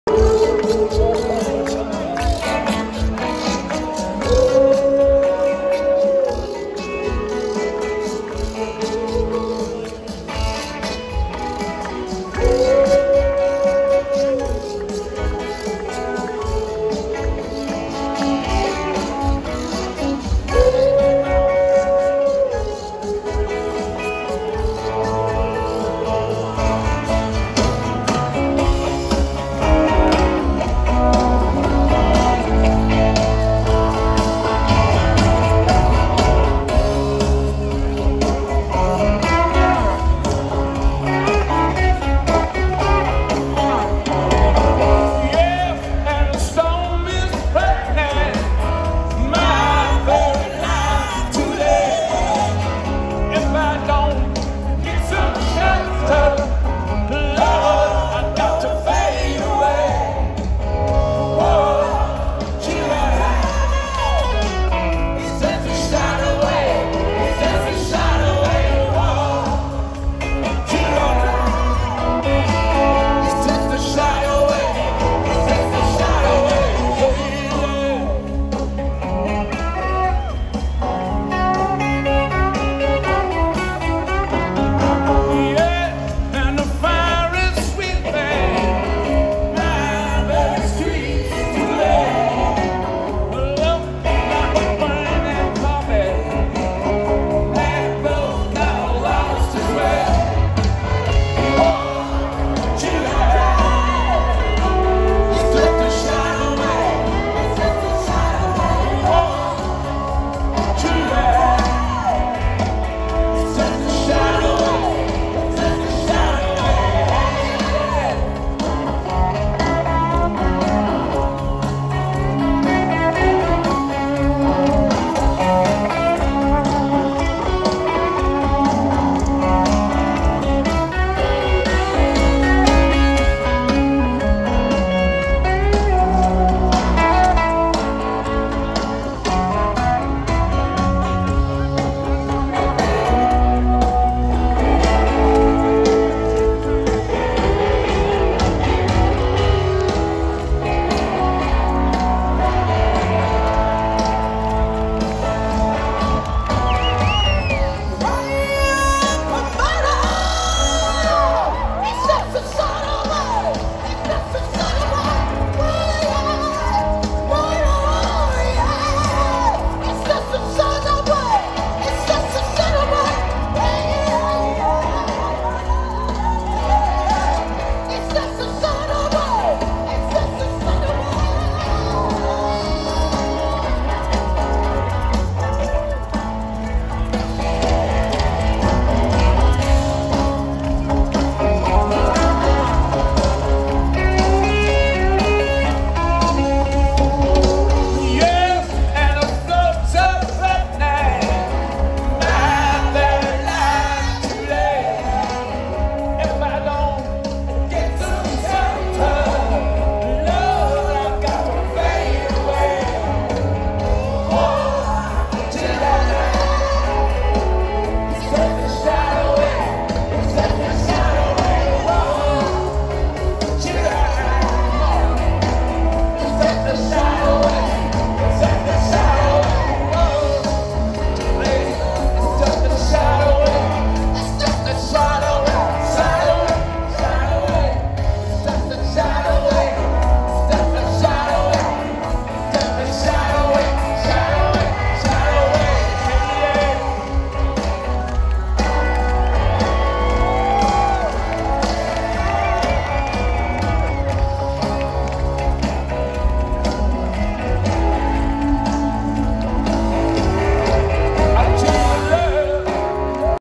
The Rolling Stones live at Hyde Park London
The rolling stones live at Hyde park 3rd July 2022 as part of there Sixty tour.